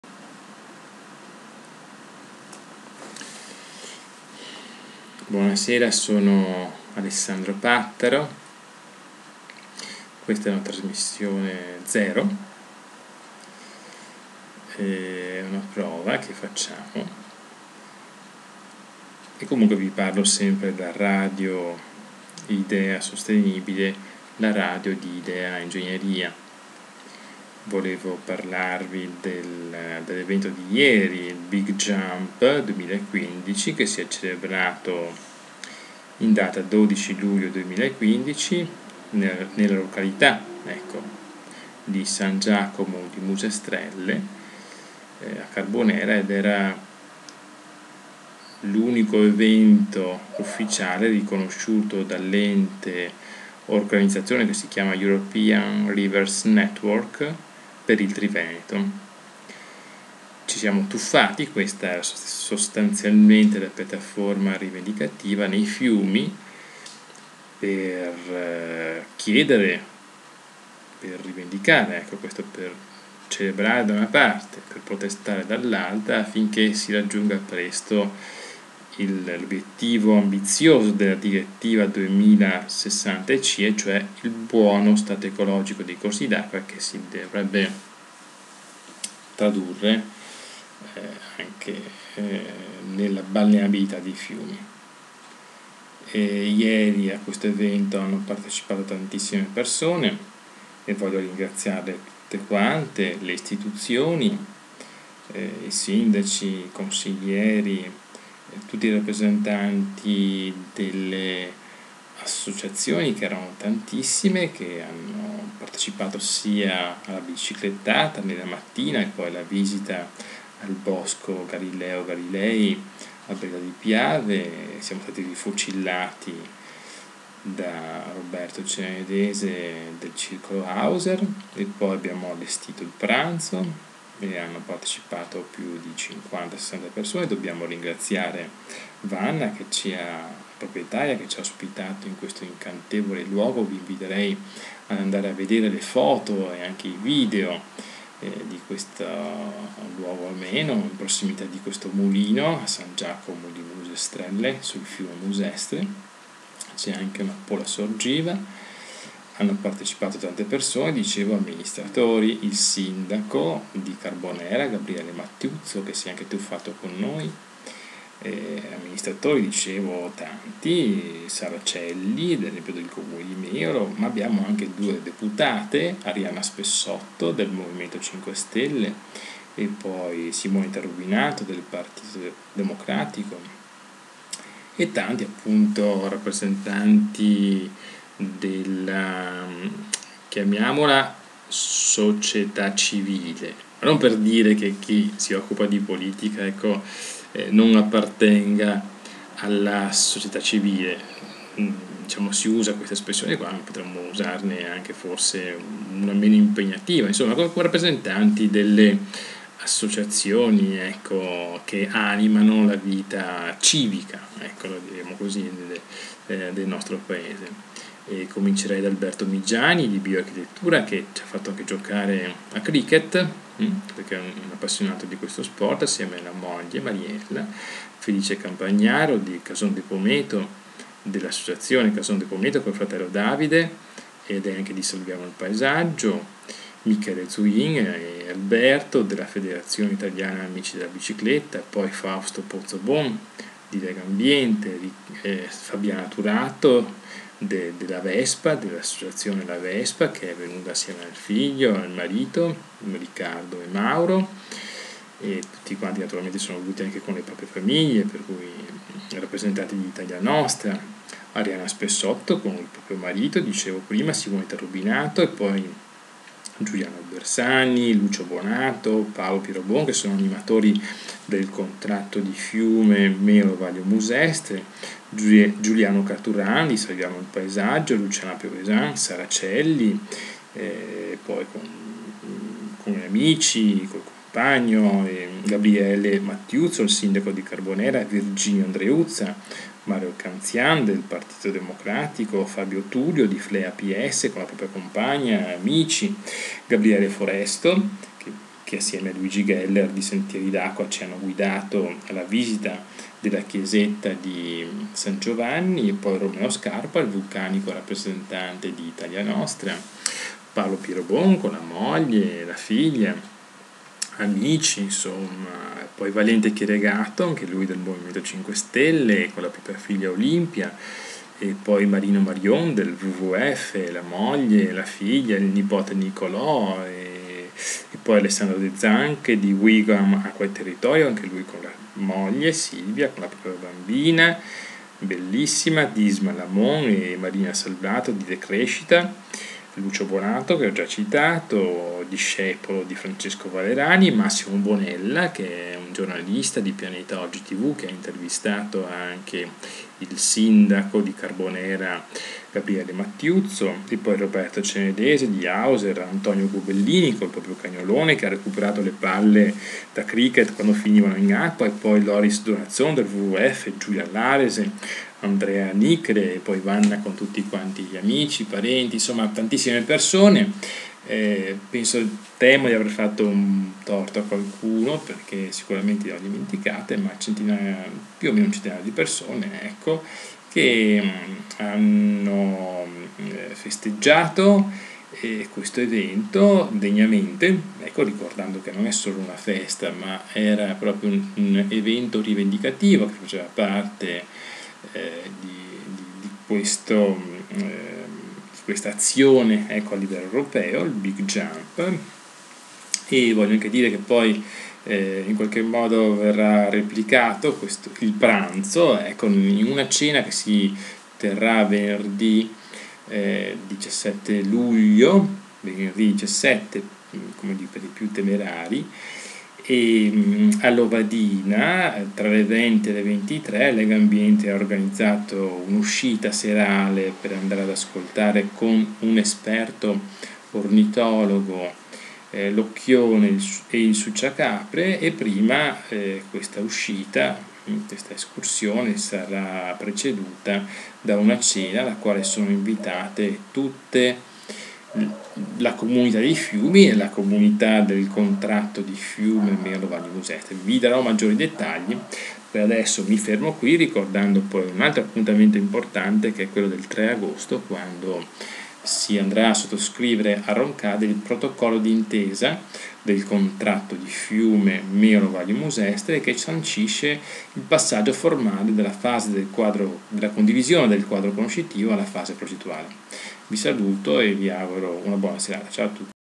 5' video test sul Big Jump e Contratto di Fiume
video test sul Big Jump 2015 celebrato a Carbonera il 12 Luglio e sulle attività del Contratto di Fiume Meolo Vallio Musestre